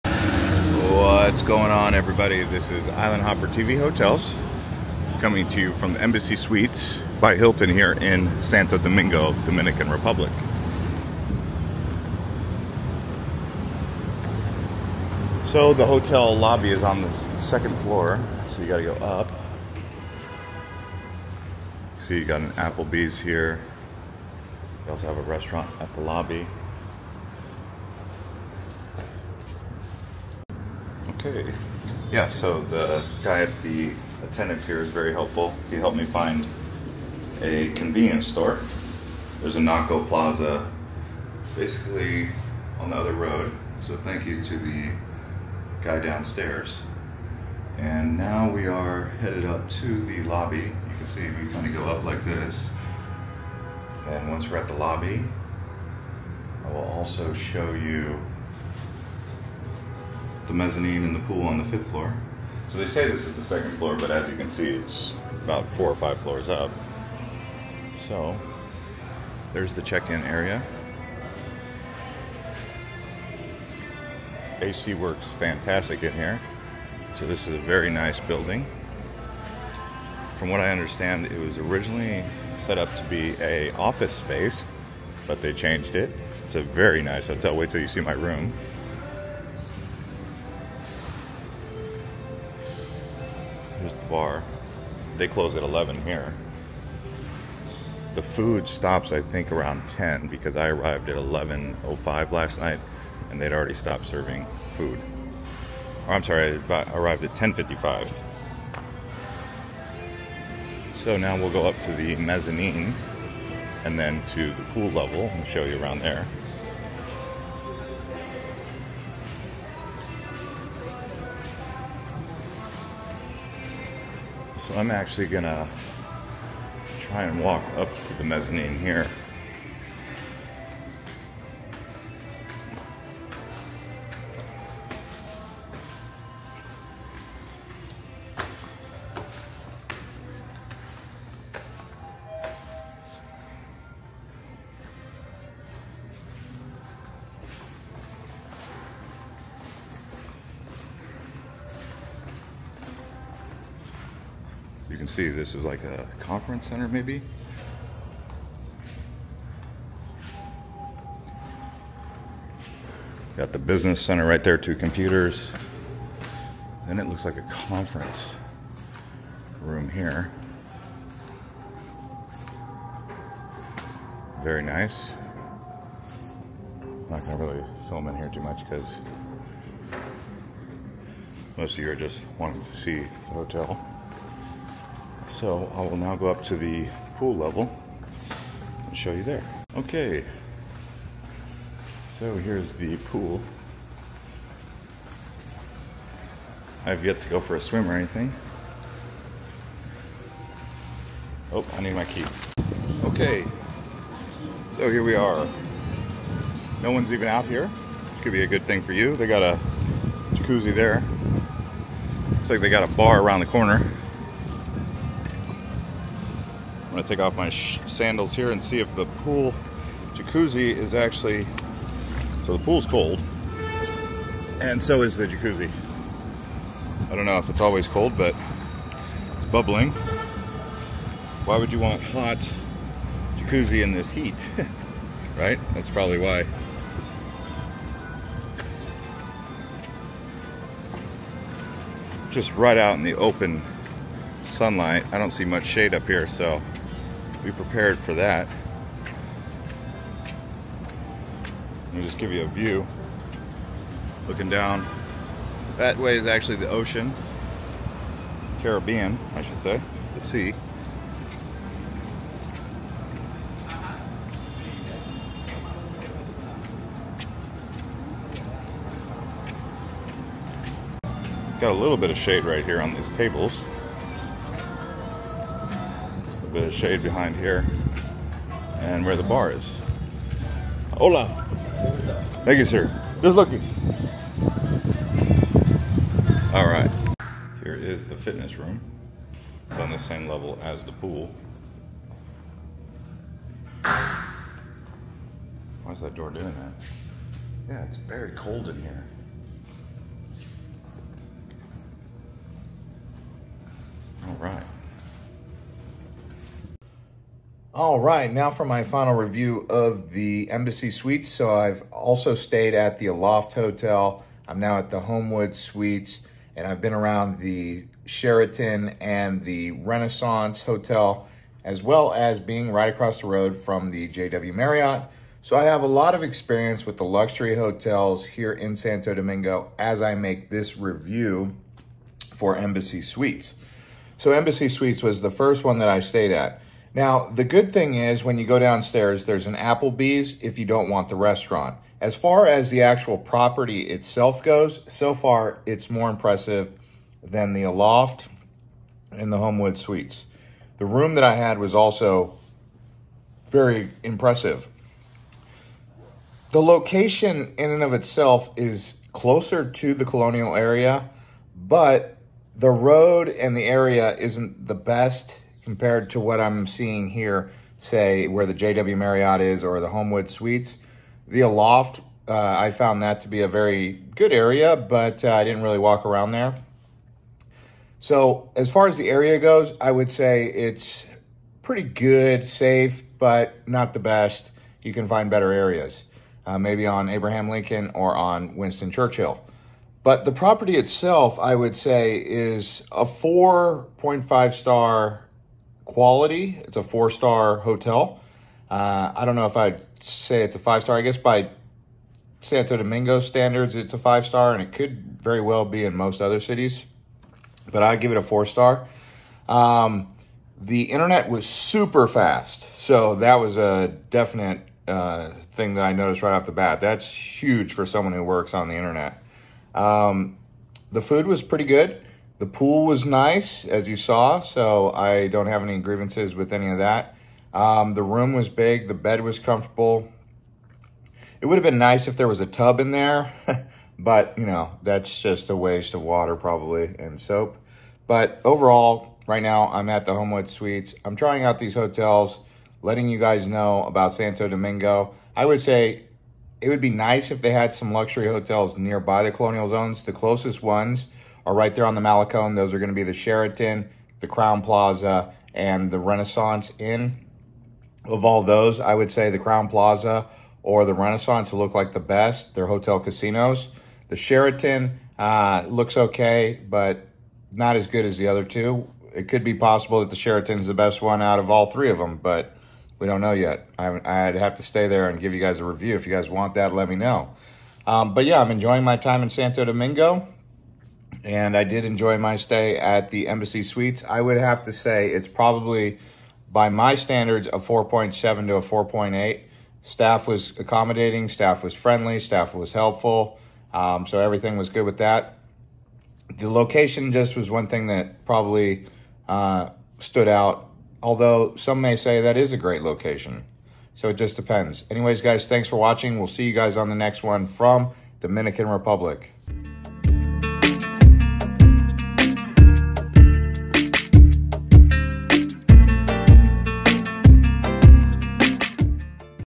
Embassy Suites by Hilton Santo Domingo Tour 2024
This is a tour of the Embassy Suites by Hilton Santo Domingo where we give an honest review of the hotel. I rate this as a 4.7 star out of 5 star hotel. The location is the only thing really holding it back from a higher rating. The quality of the property and hotel is very good.

Island Hopper TV Hotels and Resorts is a hotel review channel that shows you the hotels and resorts from around the world.